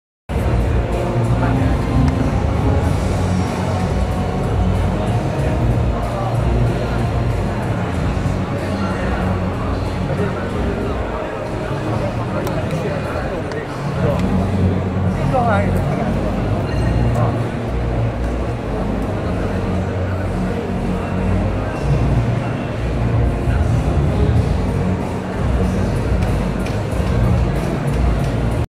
The robotic crane system RB12CR work with teleprompter demonstration
Check out the video of RB12CR robotic crane system.
It's jib length range is 2.6m-4.2m, which can be adjusted manually. Designed for OB van and studio applications to realize the camera shooting automation. A teleprompter can work together with the crane system.